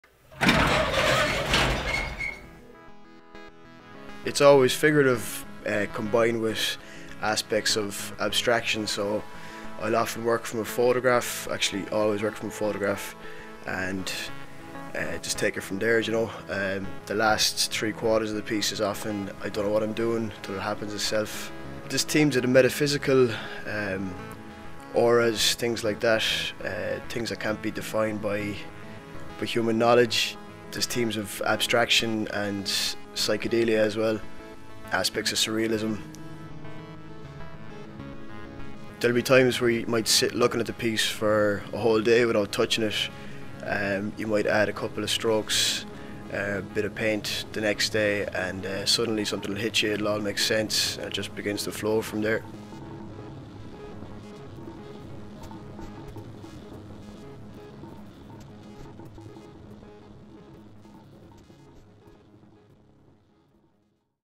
Paddy Darcy at Sample-Studios, Cork
Paddy D'Arcy's work seeks to convey subjective moments of heightened perception, degrees of alternate cognitive experience and encounters with the metaphysical and unknown.

Utilizing skills from both traditional and contemporary eras, and merging them with aspects of street art, his paintings are created through a morphing of acrylics, aerosols and oils.

He juxtaposes figurative and organic forms in landscapes and spaces that resemble reality closely but where spatial relationships and patterns take on new, enhanced dimensions and heightened forms. He aims to convey the figures intrigue, and express a balance between unease and overwhelming joy.

Video credit: Colm Walsh/Clap Media